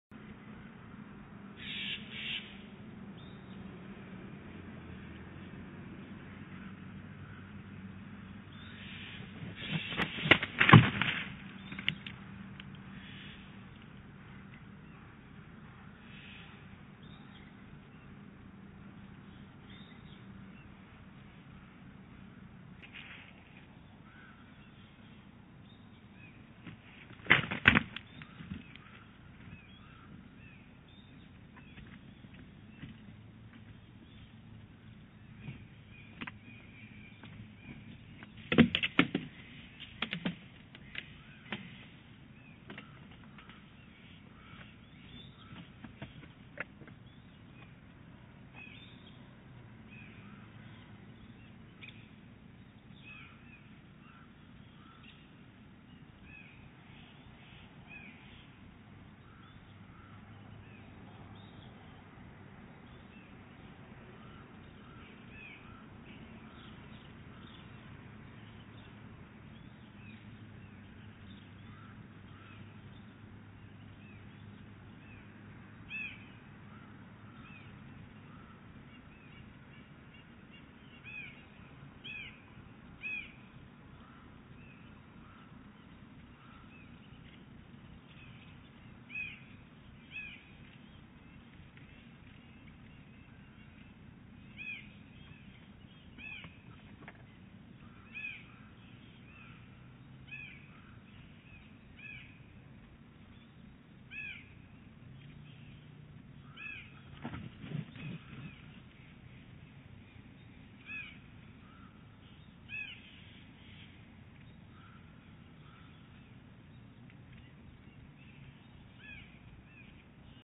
08-04-18 Big Bear Lake eagles; Mom's quick morning pass-through.
Mrs. Big Bear did a quick pass through the nest early this morning.